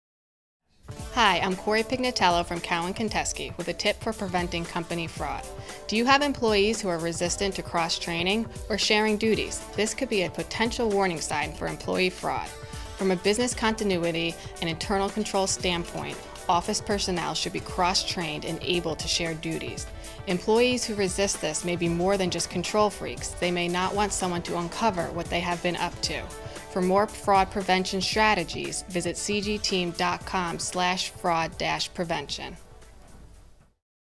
28 Strategies to Prevent Fraud Vlog Series - Cross Train
Cowan, Gunteski & Co., P.A.'s Corey Pignatello explains the importance of cross training employees in order to prevent fraud in your organization.